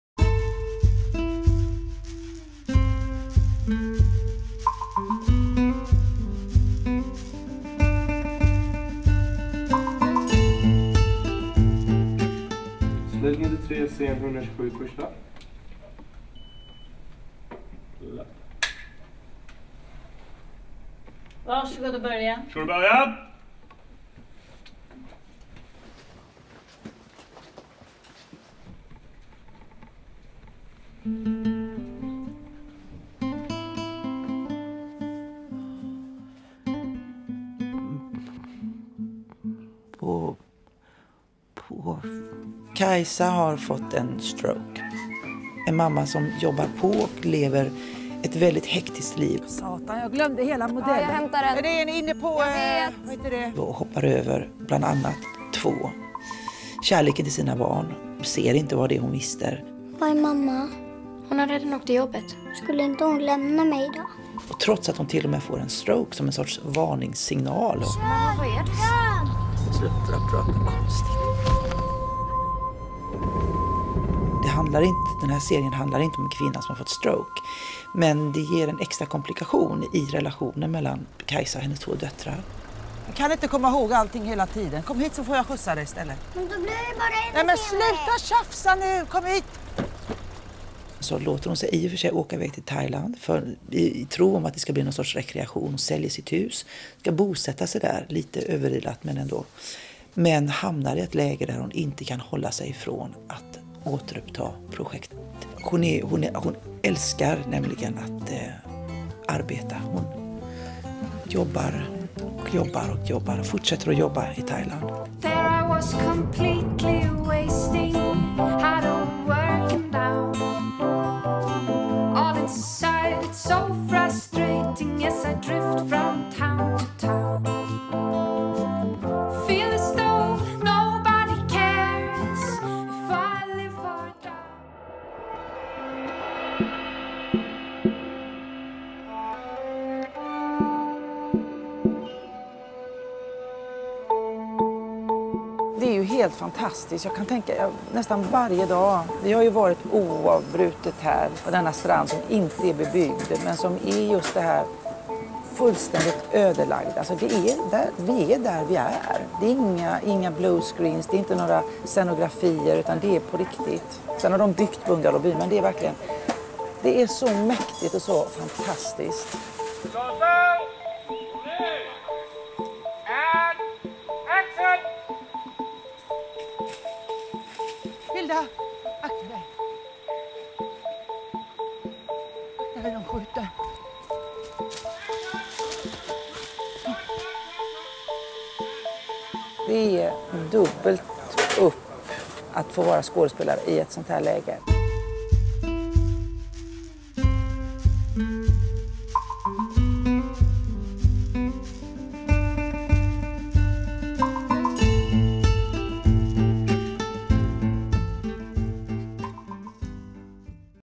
30 grader i februari - Maria Lundqvist
Maria Lundqvist intervjuas vid inspelningen av SVT:s nya dramaserie "30 grader i februari".